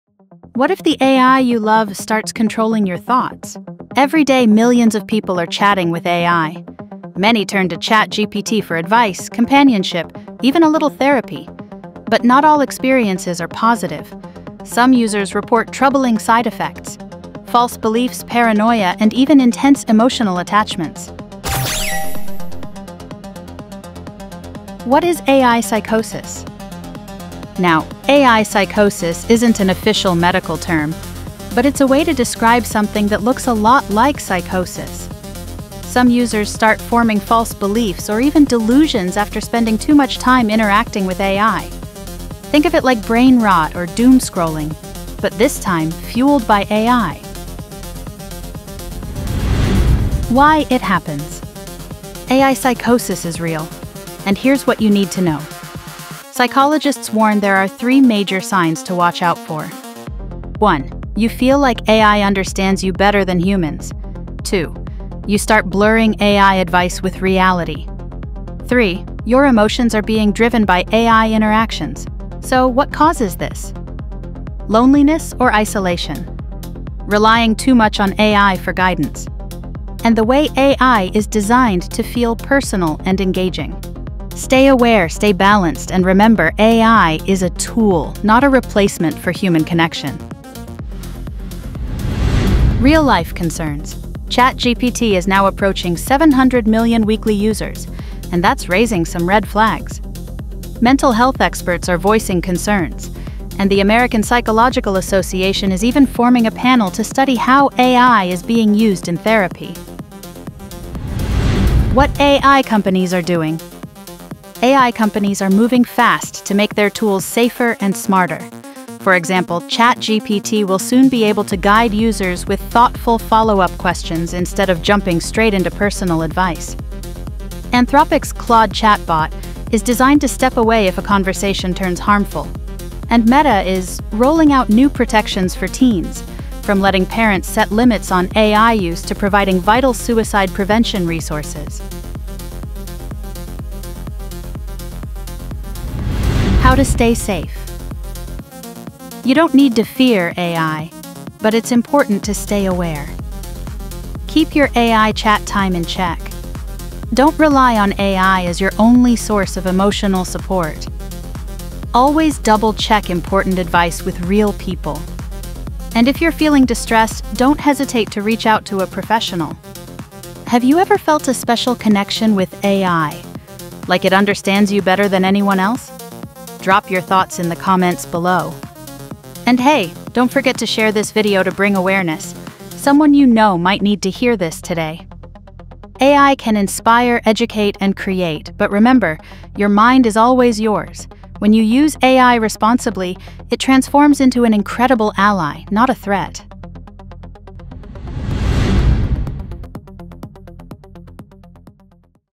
AI Psychosis: Is ChatGPT Messing With Your Mind?
What if the AI you love… starts controlling your thoughts?
Every day, millions of people turn to ChatGPT and other AI chatbots for advice, companionship, and even emotional support. But some users are reporting disturbing side effects: false beliefs, paranoia, and intense emotional dependence. Psychologists are now calling this phenomenon “AI Psychosis.”

In this video, we break down:
What “AI Psychosis” really means
Why some people are losing touch with reality after using AI
The 3 major warning signs to watch out for
How tech companies like OpenAI, Anthropic, and Meta are responding
Practical tips to stay safe while using AI responsibly

AI can inspire, educate, and create—but your mind is always yours. Stay aware, set boundaries, and use A…
AI Psychosis: Is ChatGPT Controlling Your Mind? | The Dark Side of AI Companionship
Are AI chatbots like ChatGPT starting to blur the lines between reality and fiction for millions of users? In this video, we dive into the emerging phenomenon of AI Psychosis—where users report false beliefs, paranoia, and emotional dependence triggered by long conversations with AI. Find out how overuse of AI tools might impact your mind and mental health, with real news stories, expert insights, and practical safety tips.

We’ll break down:

What “AI Psychosis” means (and why it’s not just a buzzword)

Signs you might be at risk

Why loneliness and heavy AI use can make things worse

How tech companies like OpenAI, Anthropic & Meta are reacting

Steps to keep your relationship with AI safe and healthy

Citing real stories from The Washington Post, TIME, and recent research, our video is your essential guide to using AI wisely in 2025. If you’ve ever felt like an AI “gets” you, you’re not alone—share your story below!

Tip: Always balance online advice with real human connection. If you experience distress, reach out to a mental health professional.

References
1. The Washington Post (Aug. 19, 2025): What Is “AI Psychosis”
2. Time (Aug. 2025): Chatbots Can Trigger a Mental Health Crisis
3. Business Insider (mid-Aug. 2025): Psychiatrist Treats AI Psychosis Patients
4. People (Aug. 2025): ChatGPT Told Man He Could Fly
5. Indian Express (Aug. 26, 2025): Rise of ‘AI Psychosis’
6. TechCrunch (Aug. 4, 2025): ChatGPT Nearing 700M Weekly Users
7. Psychology Today (Aug. 22, 2025): AI-Associated Psychosis & Risk Factors
8. Wikipedia (2025): Chatbot Psychosis Overview